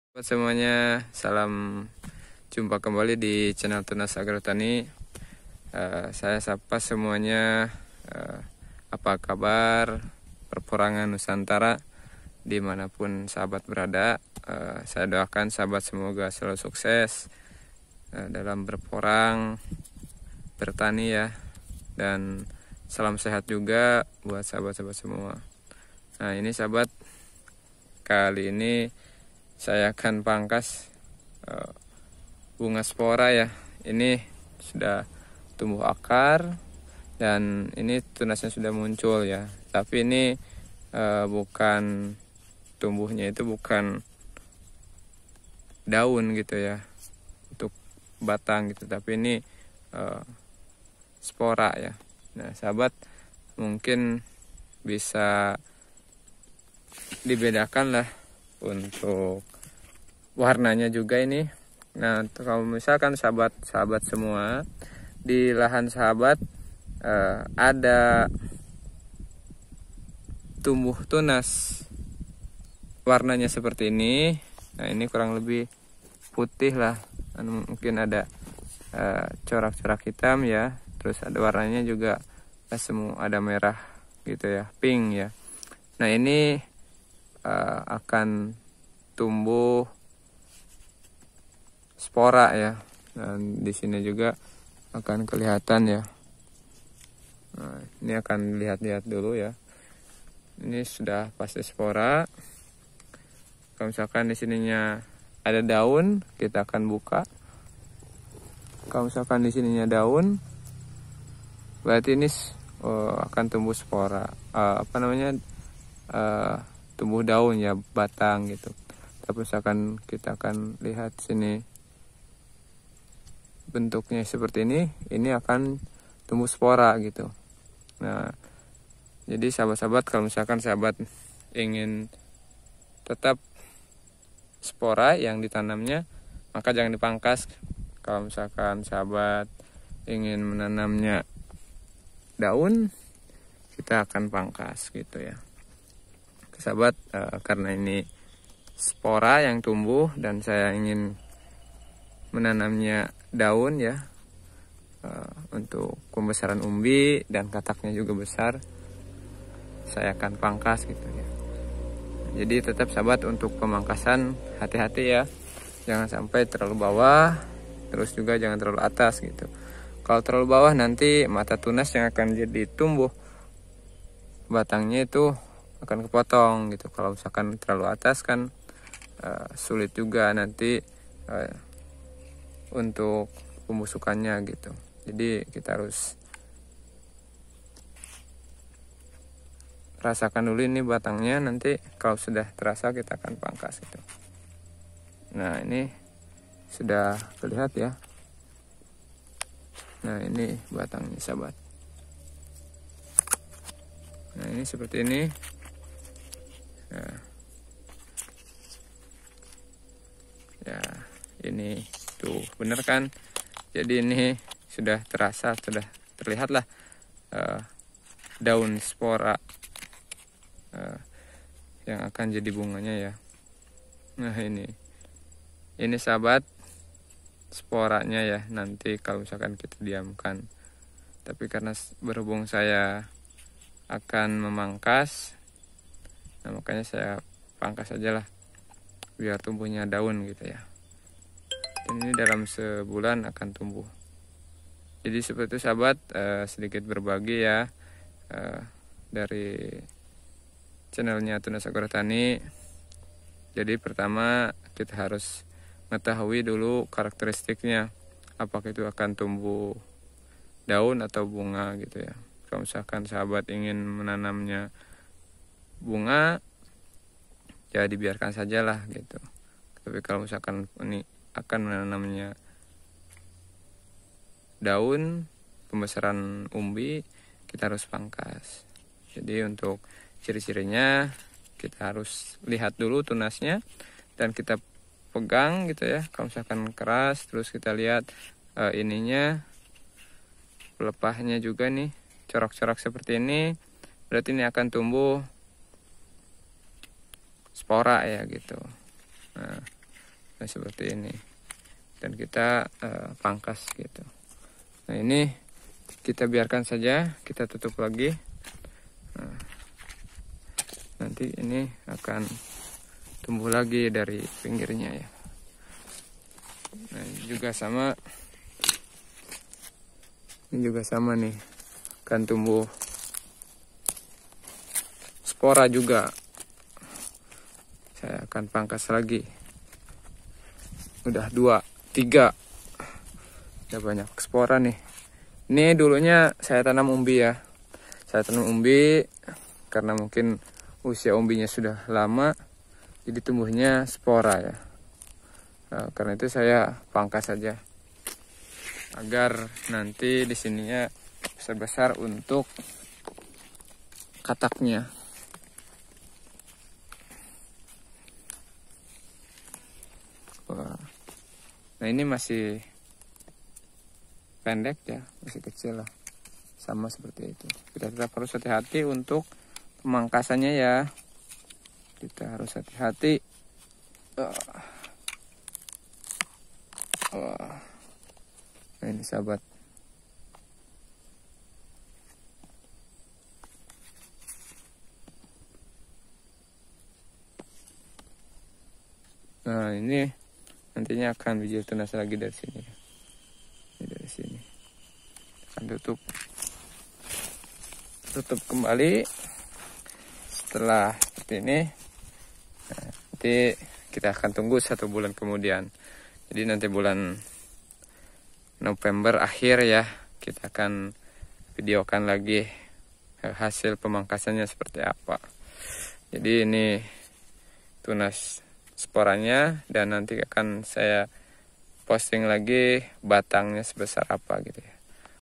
Ciri-ciri Porang Tumbuh Bunga Acung Dan Daun
pertumbuhan Porang ada dua macam,
yang pertama yaitu Porang tumbuh daun,
dan yang kedua adalah tumbuh Bunga Acung(SEPORA)